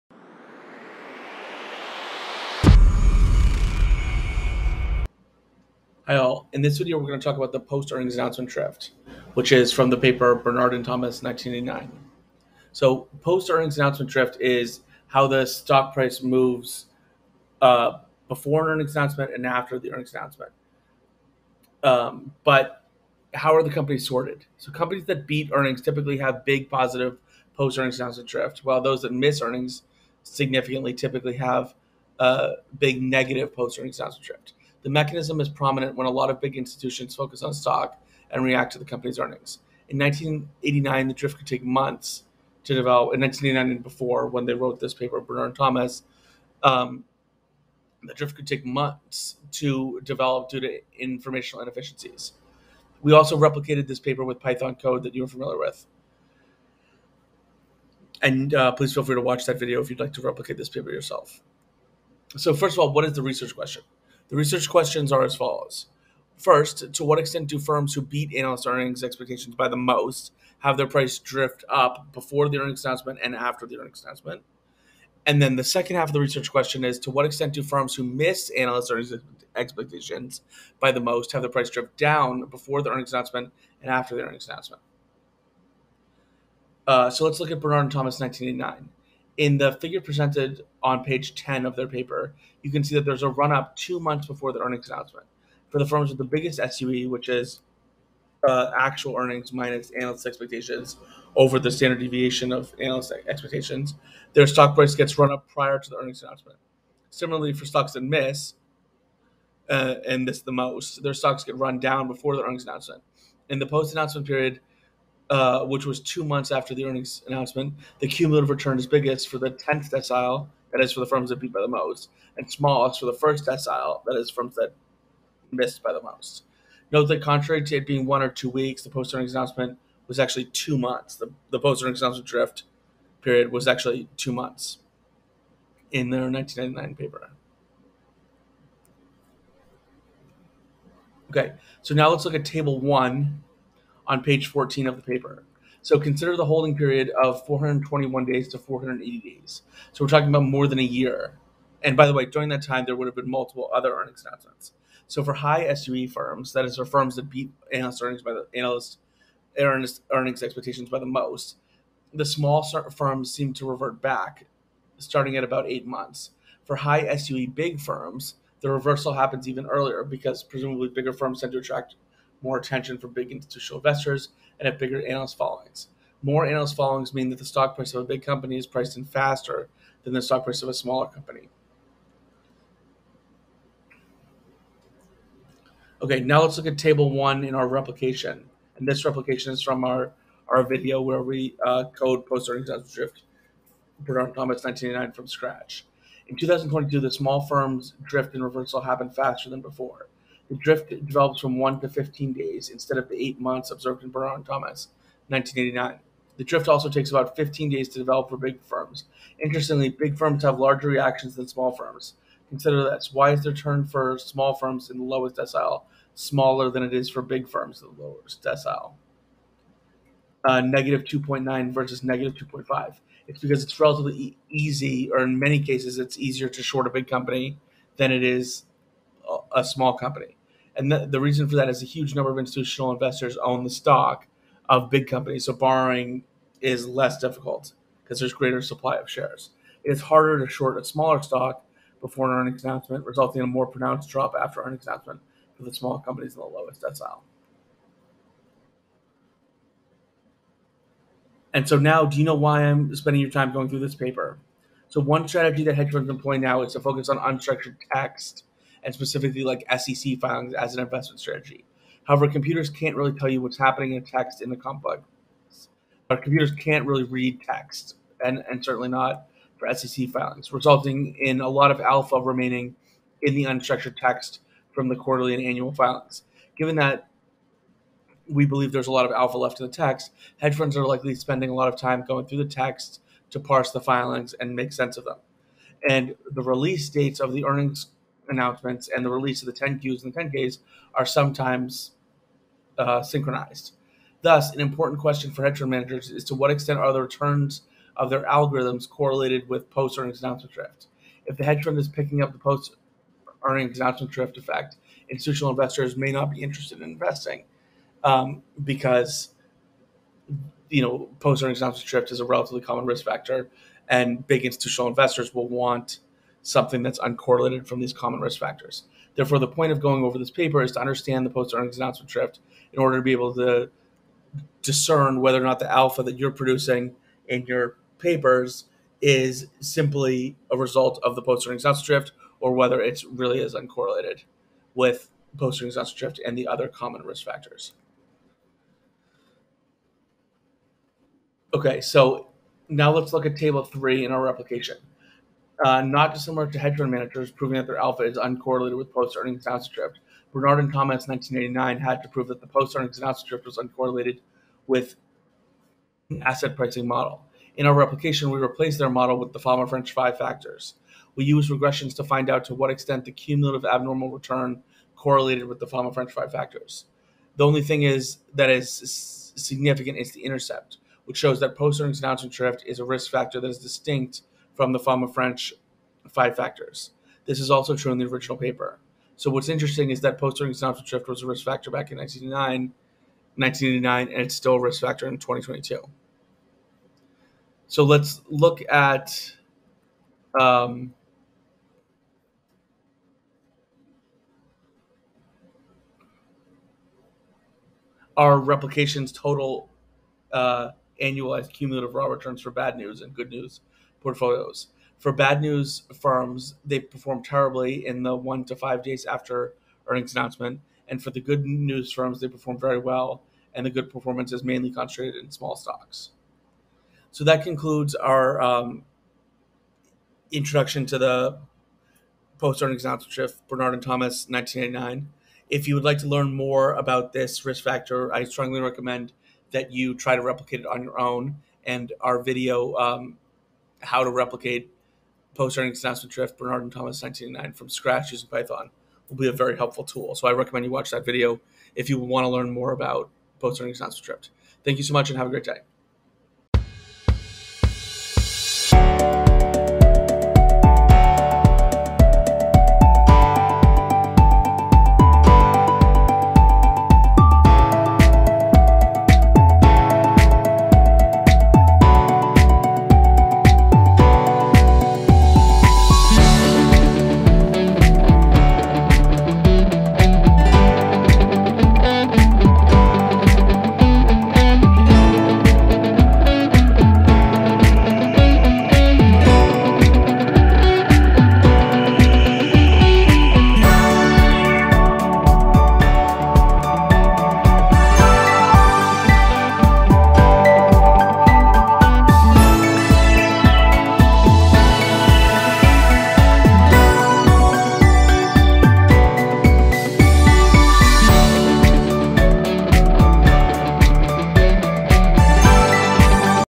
What is Post-Earnings Announcement Drift?
In this video, I will introduce you to the Post-Earnings Announcement Drift.